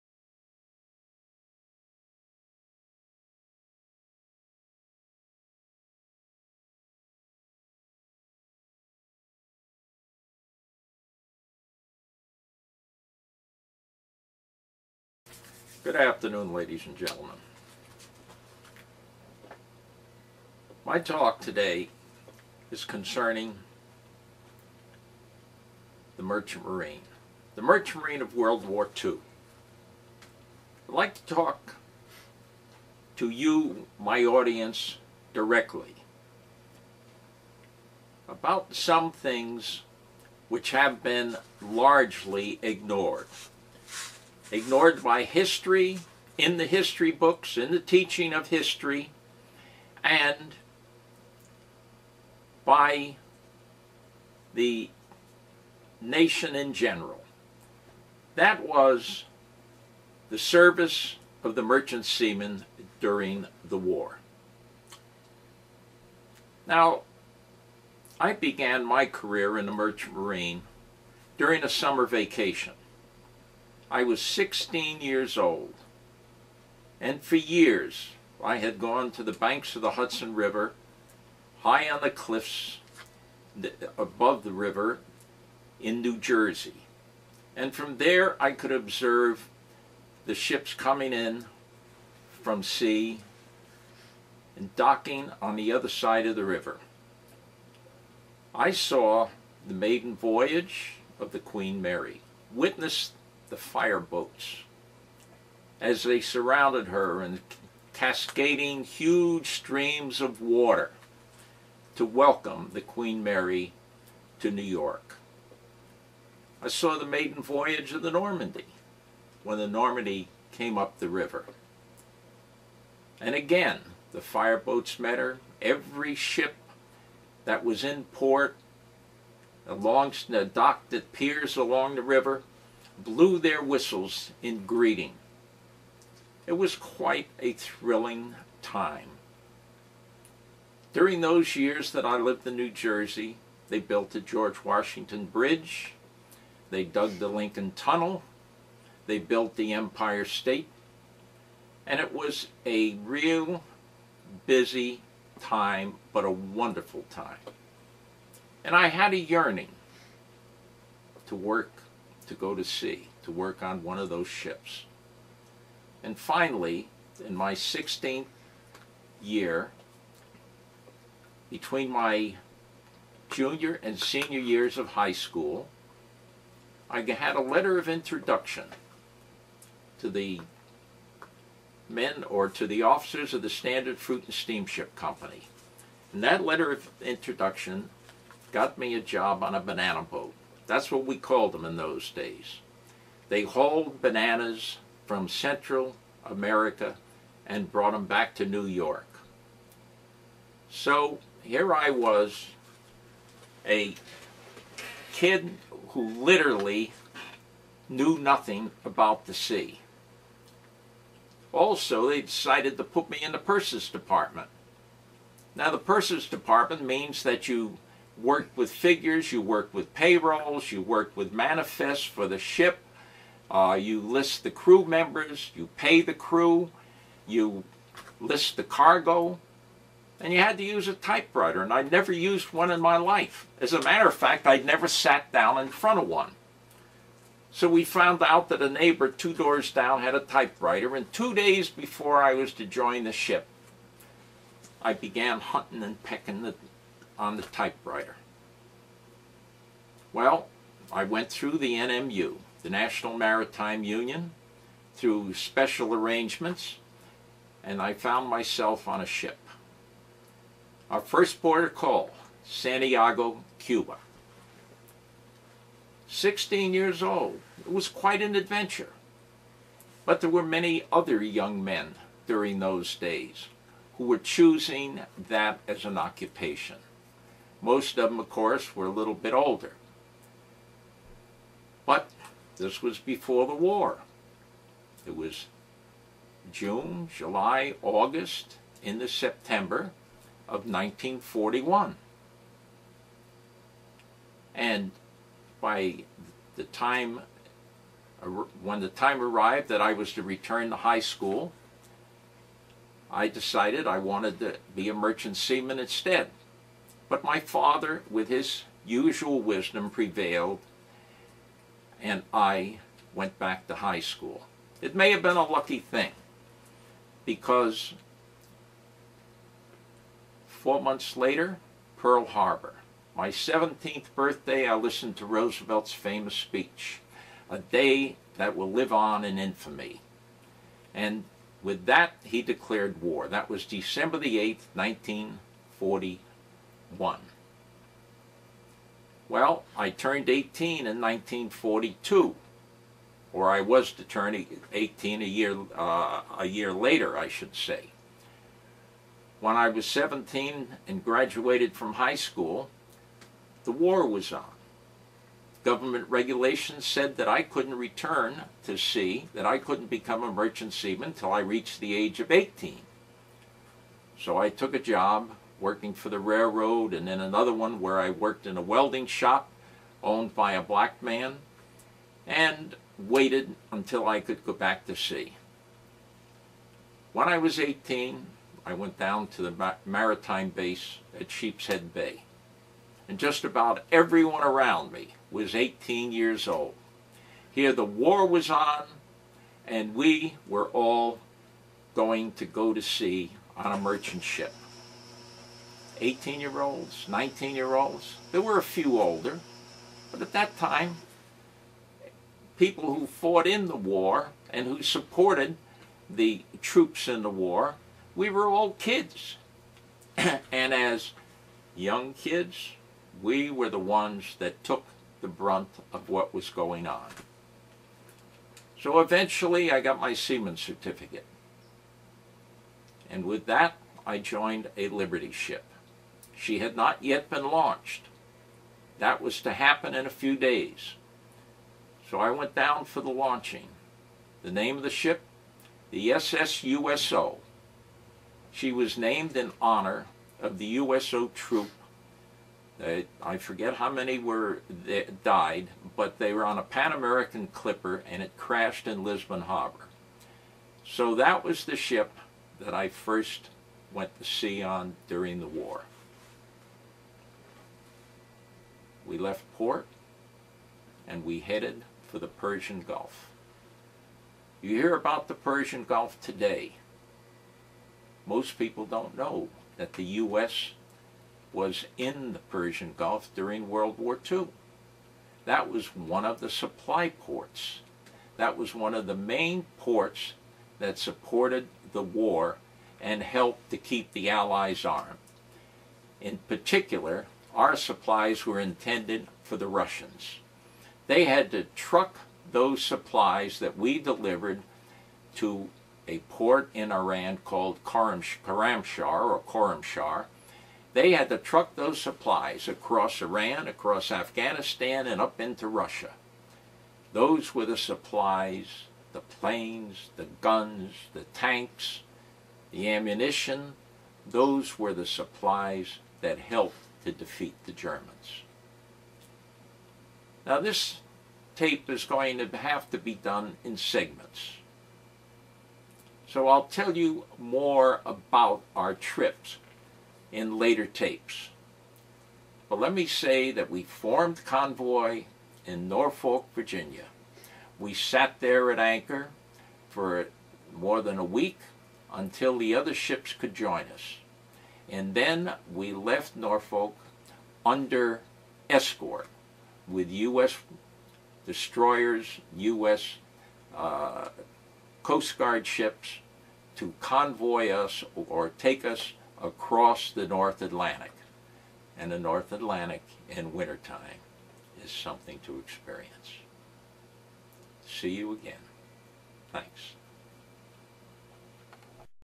Merchant Marine during World War II
"Going to Sea" at 16 in the summer of 1941, the start of WWII in December 1941, and then, convoys crossing the North Atlantic.